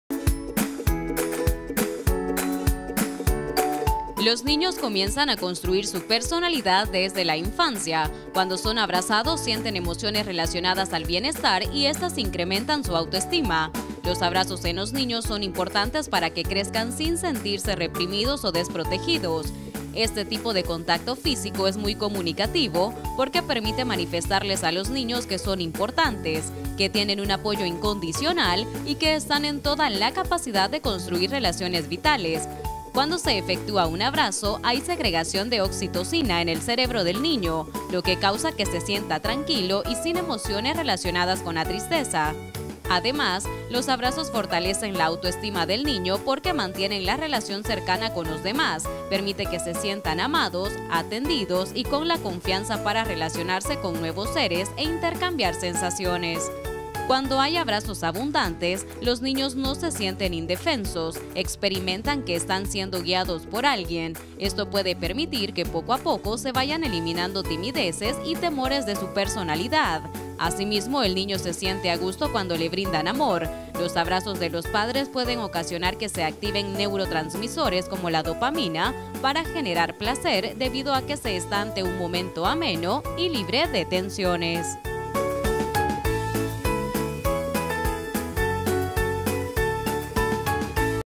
DeMujerAMujer | ¿Por qué son importantes los abrazos para los niños?
¿Alguna vez te imaginaste cuáles pueden ser los beneficios de abrazar a los niños? Respondemos a esta pregunta a través de la siguiente nota